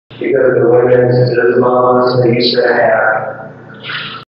Jail 26
Sandusky County Jail, Dungeon & Gallows- Audio: This was taken 34m54s into the recording in the Jail on my Digital Recorder; during the Ghost Radio being on. "Because the Womans was just above us they used to have..." Response: "No" at 3.5s in.  **this was directly following him turning off the Ghost Radio and start talking.